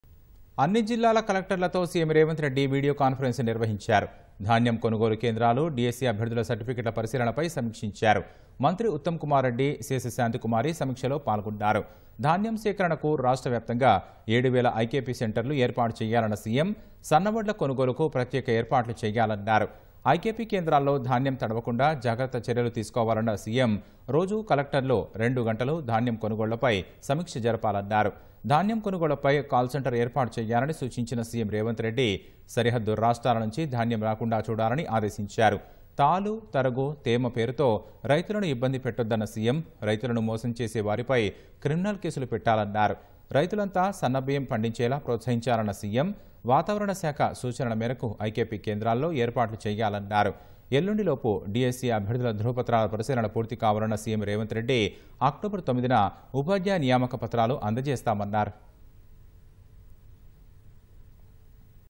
CM Revanth Reddy Review on Paddy Purchase Centers and DSC Certificate Verification | సీఎం సమీక్ష
అన్ని జిల్లాల కలెక్టర్లతో సీఎం రేవంత్ రెడ్డి వీడియో కాన్ఫరెన్స్  నిర్వహించారు.  ధాన్యం కొనుగోలు కేంద్రాలు, డీఎస్సీ అభ్యర్థుల సర్టిఫికెట్ల పరిశీలనపై సమీక్షించారు. మంత్రి ఉత్తమ్ కుమార్ రెడ్డి, సీఎస్ శాంతి కుమారి సమీక్షలో పాల్గొన్నారు.  ధాన్యం సేకరణకు రాష్ట్రవ్యాప్తంగా 7 వేల ఐకేపీ సెంటర్లు ఏర్పాటు చేయాలన్న సీఎం.. సన్న వడ్ల కొనుగోలుకు ప్రత్యేక ఏర్పాట్లు చేయాలన్నారు.  ఐకేపీ కేంద్రాల్లో ధాన్యం తడవకుండా జాగ్రత్త చర్యలు తీసుకోవాలన్న సీఎం... రోజూ కలెక్టర్లు 2 గంటలు ధాన్యం కొనుగోళ్లపై సమీక్ష జరపాలన్నారు.
ధాన్యం కొనుగోళ్లపై కాల్ సెంటర్ ఏర్పాటు చేయాలని సూచించిన సీఎం రేవంత్ రెడ్డి...  సరిహద్దు రాష్ట్రాల నుంచి ధాన్యం రాకుండా చూడాలని ఆదేశించారు. తాలు, తరుగు, తేమ పేరుతో రైతులను ఇబ్బంది పెట్టొద్దొన్న సీఎం... రైతులను మోసం చేసే వారిపై క్రిమినల్ కేసులు పెట్టాలన్నారు.  రైతులంతా సన్నబియ్యం పండించేలా ప్రోత్సహించాలన్న సీఎం...  వాతావరణశాఖ సూచనల మేరకు ఐకేపీ కేంద్రాల్లో ఏర్పాట్లు చేయాలన్నారు.  ఎల్లుండిలోపు డీఎస్సీ అభ్యర్థుల ధ్రువపత్రాల పరిశీలన పూర్తికావాలన్న సీఎం రేవంత్ రెడ్డి...  అక్టోబర్ 9న ఉపాధ్యాయ నియామకపత్రాలు అందజేస్తామన్నారు..